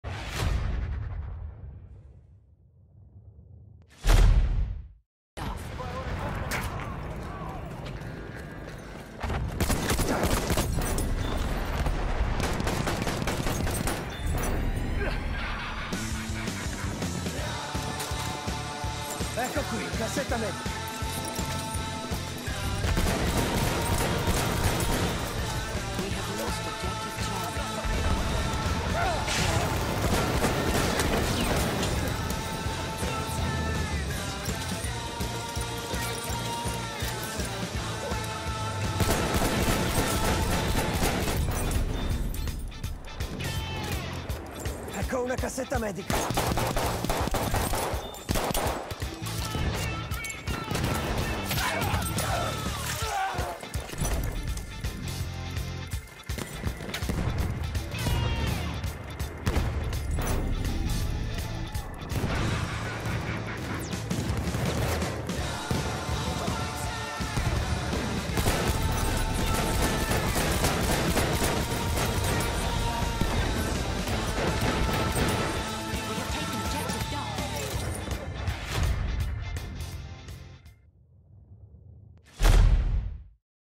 Bunker Raid
What was intended as a suicide rush turned into a slaughter fest